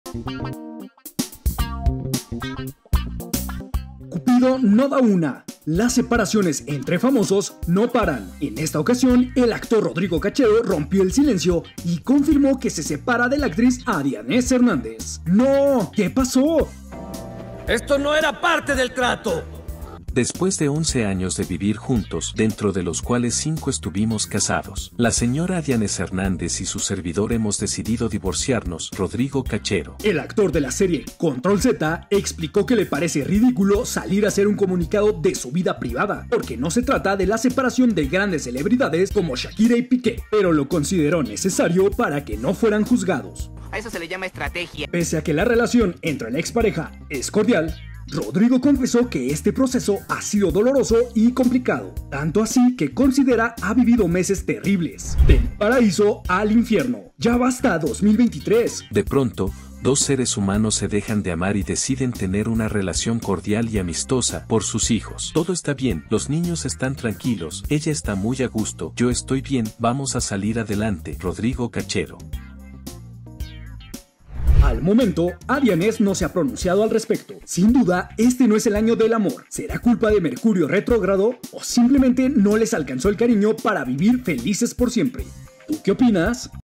Se acabó el amor entre Rodrigo Cachero y Adianez I TVNotas I Noticias
¡Otra pareja que se separa! Rodrigo Cachero y Adianez. Se especuló que el divorcio sería a causa de una infidelidad luego de que Cachero comparara la ruptura con la de Shakira y Piqué. 💔